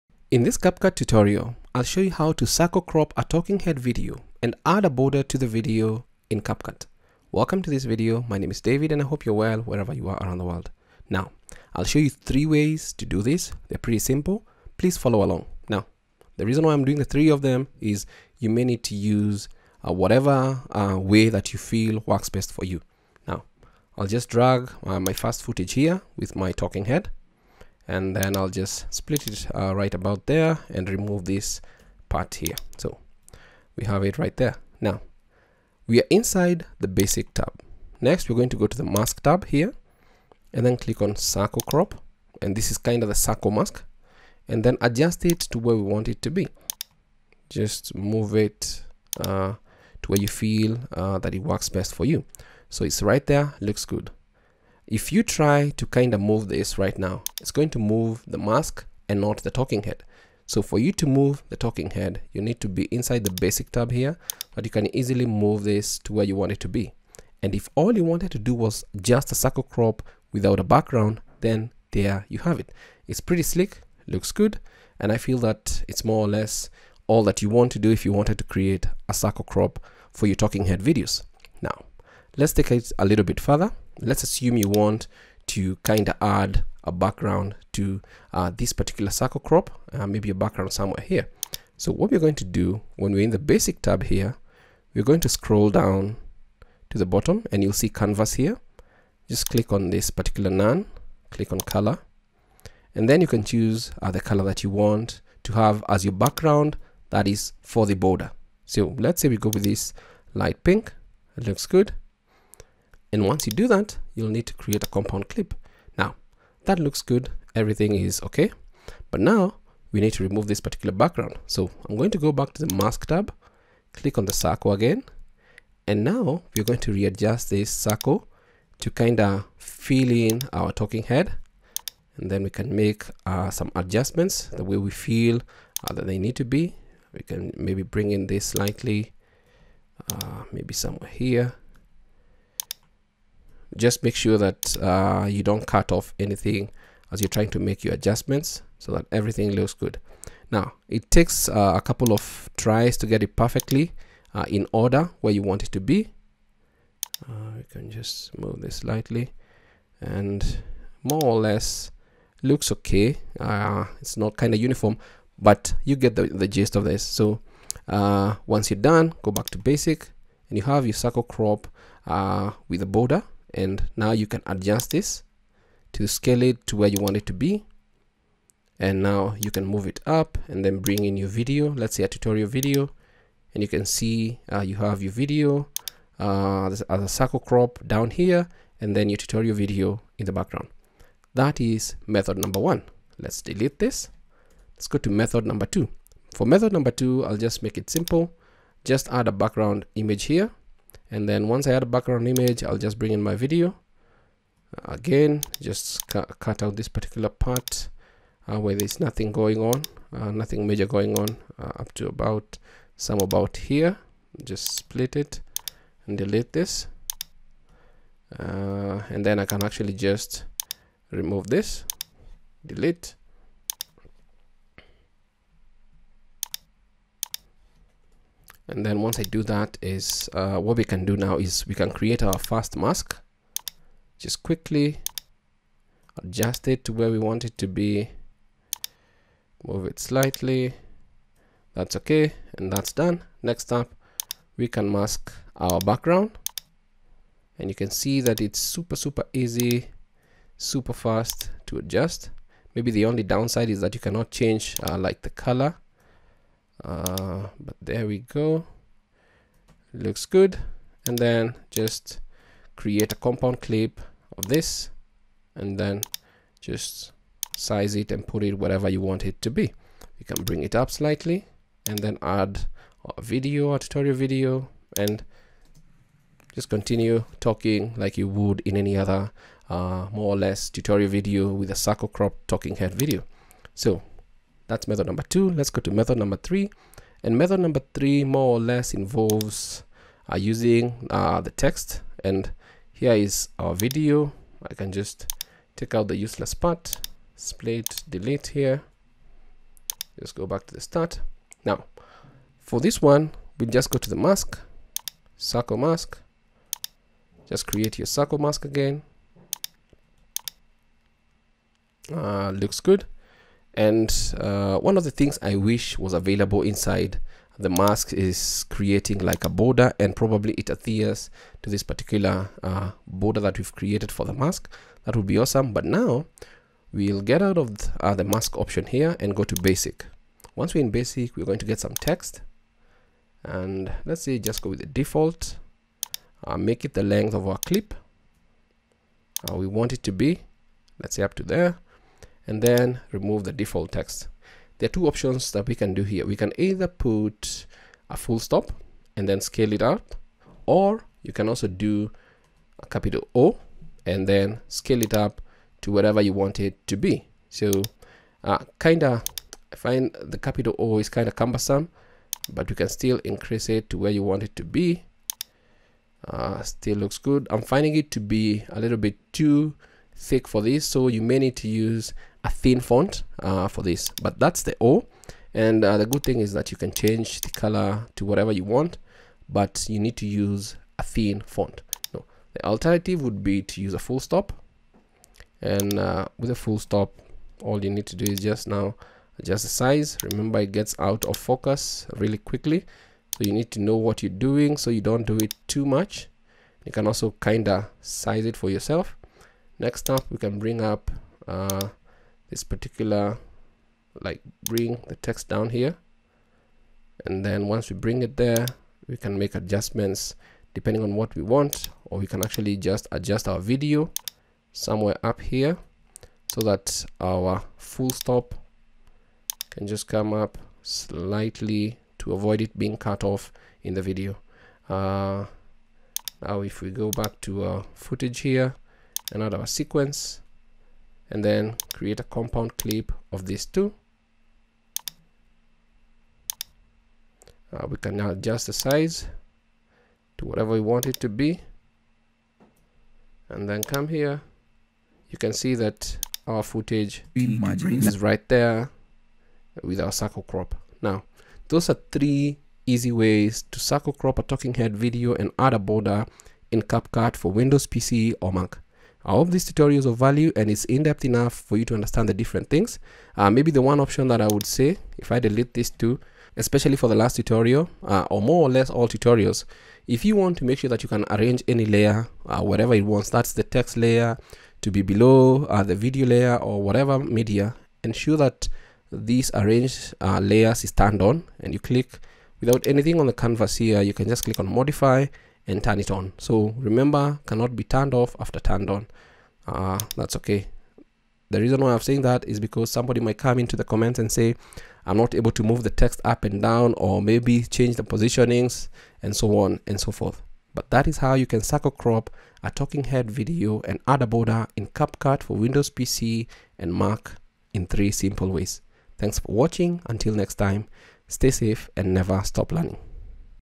How to Circle Crop a Video or Talking Head Video and Add a Border Outline Effect in CapCut
In this tutorial, I'll show you How to Circle Crop a Video or Talking Head Video and Add a Border Outline Effect in CapCut for Windows PC and MAC.

00:00 - Introduction
00:24 - Method 1
03:15 - Method 2
04:47 - Method 3
07:31 - Conclusion